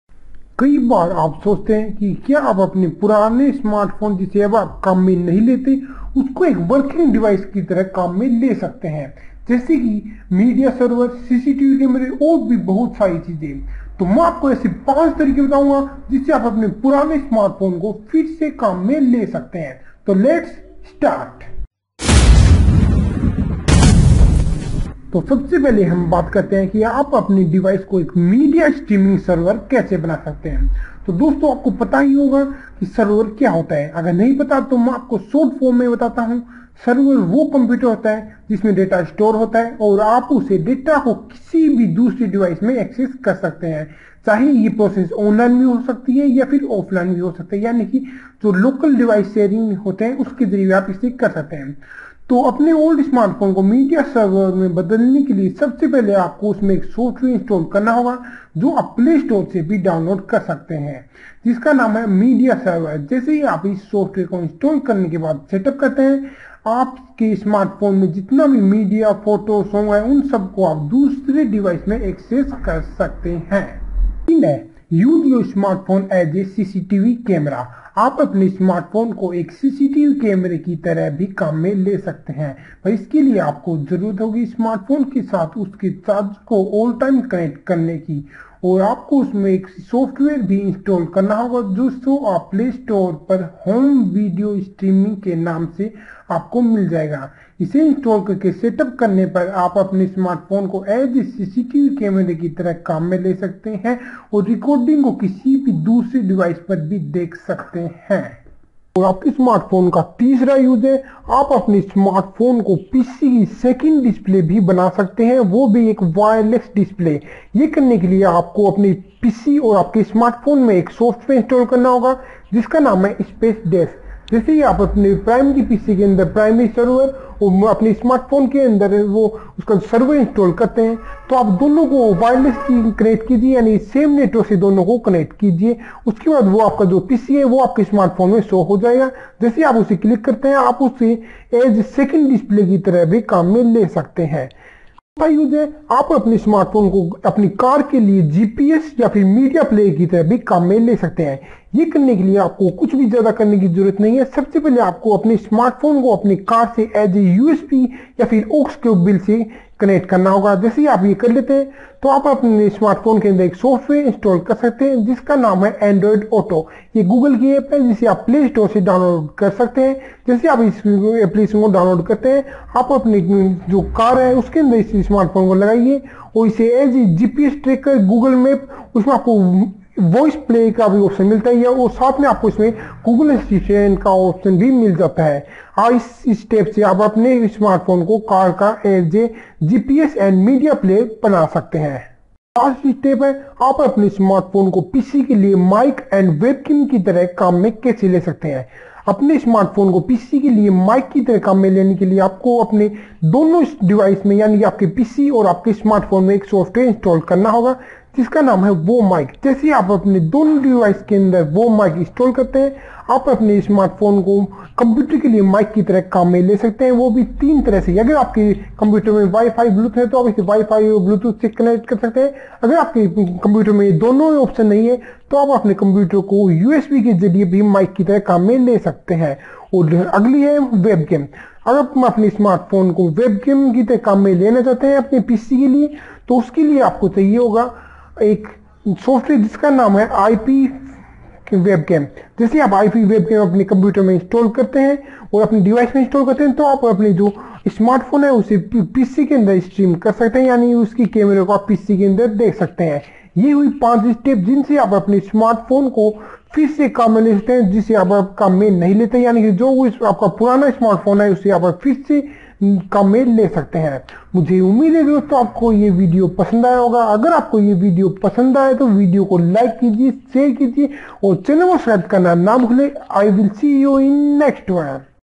Top 5 Use for your old smartphone | Detailed | Nishant Bharat
In computing, a server is a computer program or a device that provides functionality for other programs or devices, called "clients". This architecture is called the client–server model, and a single overall computation is distributed across multiple processes or devices.

2.cctv - Closed-circuit television, also known as video surveillance, is the use of video cameras to transmit a signal to a specific place, on a limited set of monitors

3.Second Monitor -At least two machines are required: PRIMARY machine and SECONDARY machine
All machines must be connected to a LAN (Local Area Network).
Network protocol TCP/IP needs to be supported. The connection can be over USB (e.g. tethering), via LAN-cable or wireless.

4.Car Gps - Android Auto is a mobile app developed by Google to mirror features from an Android device to a car's compatible in-dash information and entertainment head unit or to a dashcam

5.Mic - Mic covers news, opinion, reviews and analysis around arts, entertainment, celebrity, LGBTQ, social justice, police brutality, dating, sex, feminism, body positivity, ...